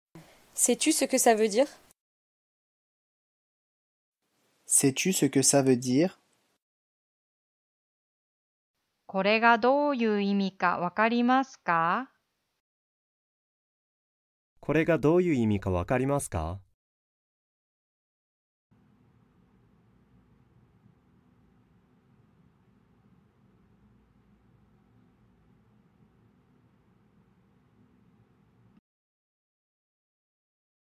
フランス語で"これがどういう意味が分かりますか？"の発音の仕方
男性コーチ
Sais-tu ce que ça veut dire ?

これがどういう意味が分かりますか？